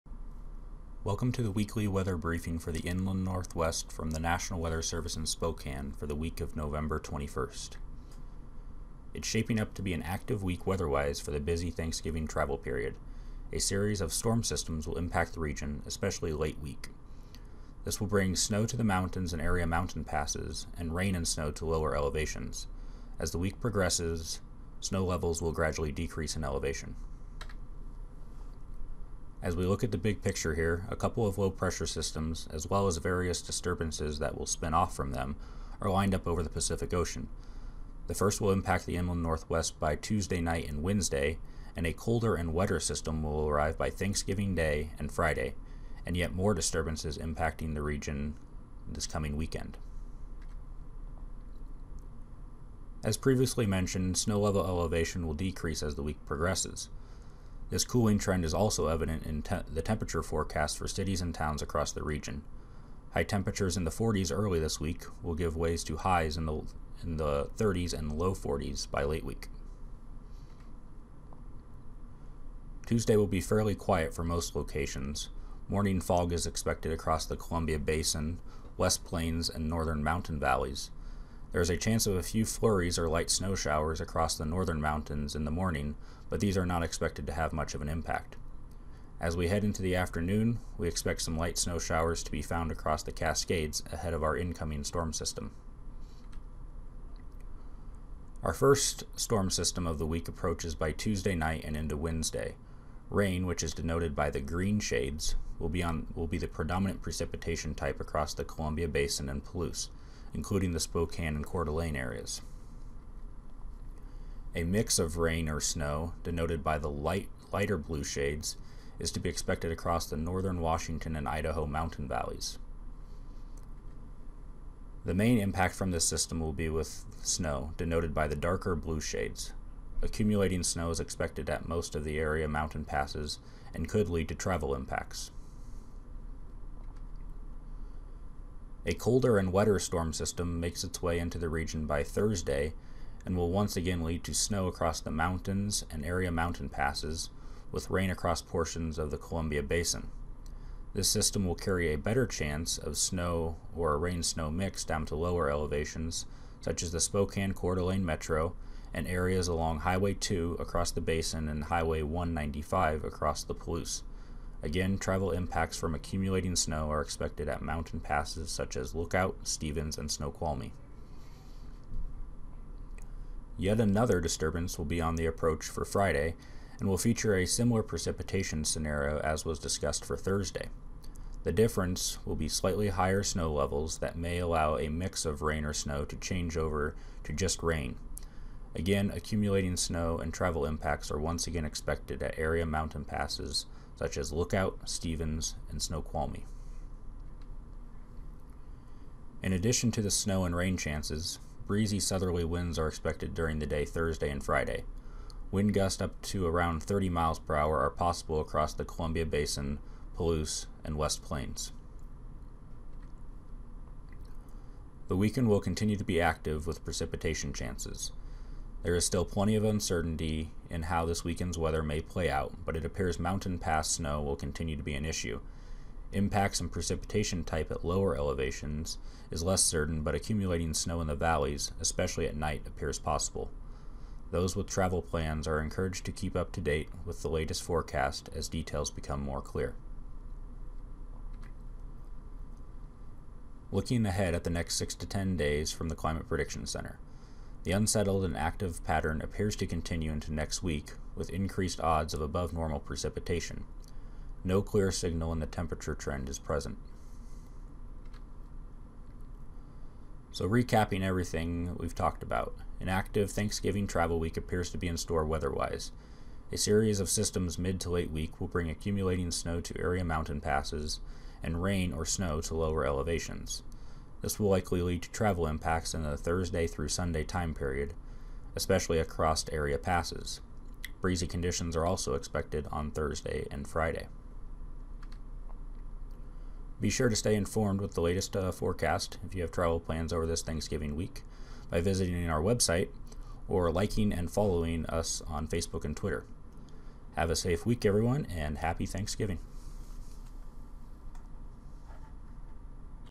Weekly Weather Briefing, Nov 21, 2016
The weekly weather briefing that will discuss the week's weather for the Thanksgiving travel period.